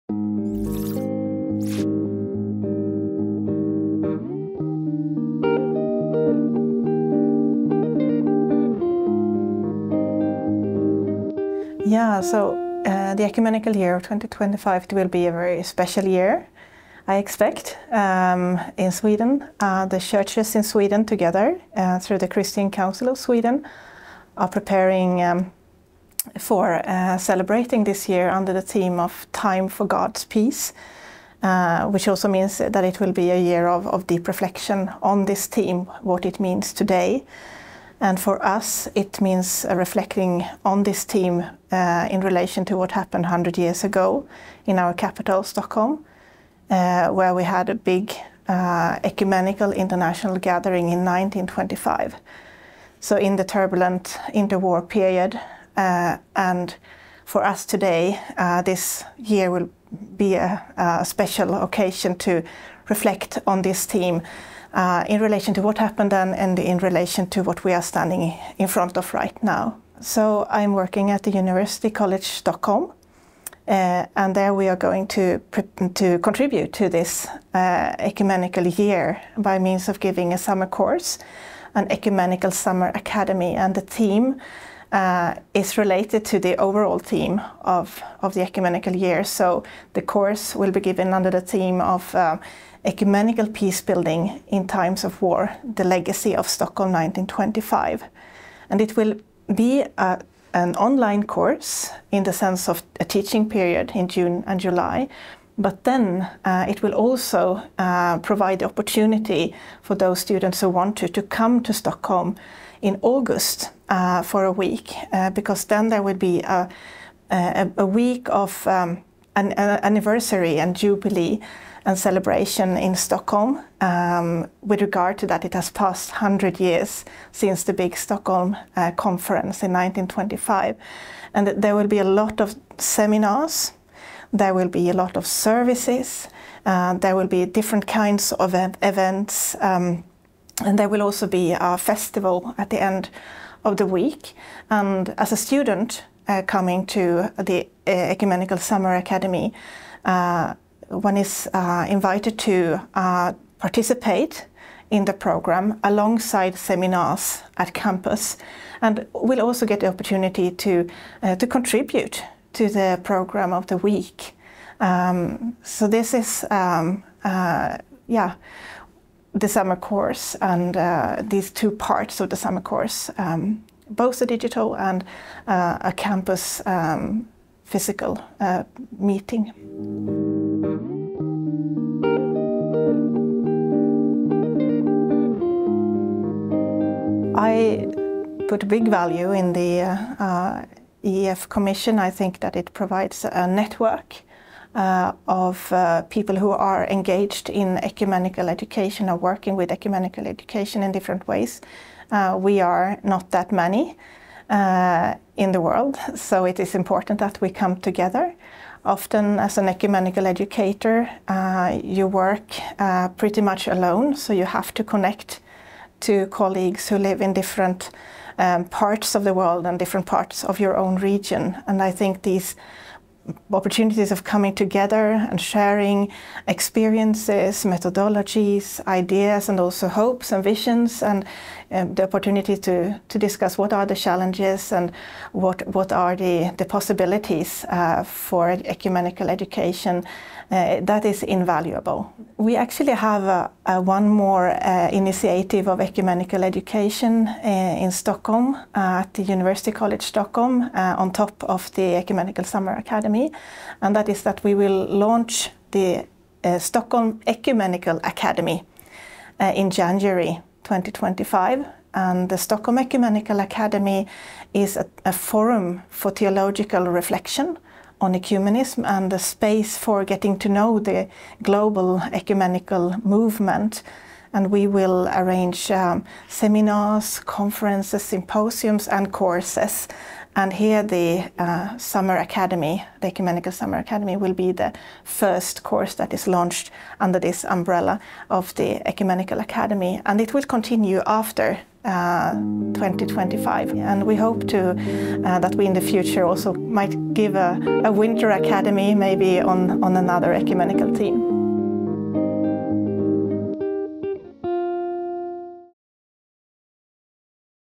WCCcommissions - Voices: Dr Sara Gehlin, Church of Sweden, University College Stockholm
In this video interview, Dr Sara Gehlin, from the Church of Sweden and the University College Stockholm, highlights what’s on the horizon, ecumenically speaking, for 2025—and why it’s special for Sweden. Gehlin is a member of the World Council of Churches (WCC) Commission on Ecumenical Education and Formation.